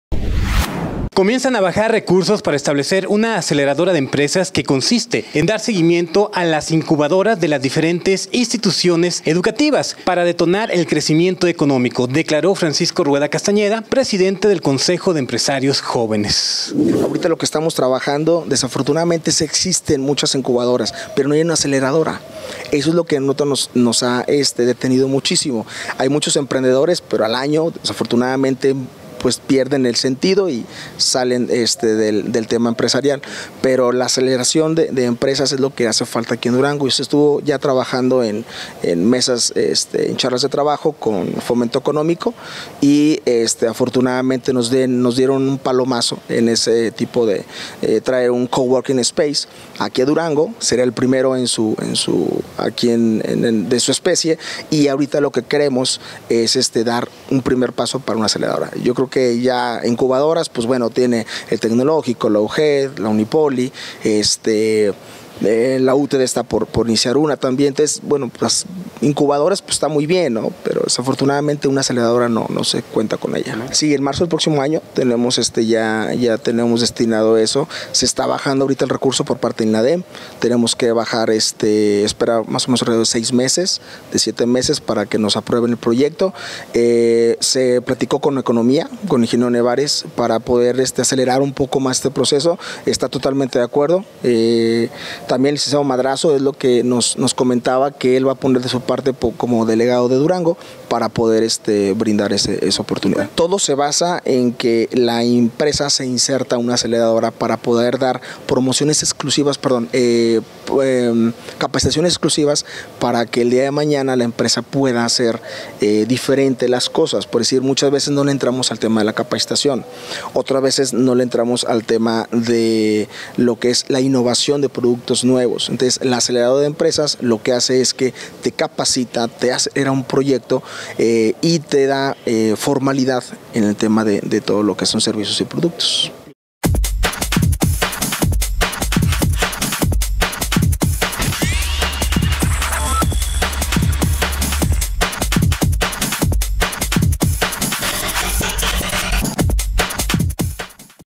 Desarrollarán una aceleradora para fomentar crecimiento económico
Comienzan a bajar recursos para establecer una aceleradora de empresas que consiste en dar seguimiento a las incubadoras de las diferentes instituciones de educación, para detonar el crecimiento económico, declaró Francisco Rueda Castañeda, presidente del Consejo de Empresarios Jóvenes.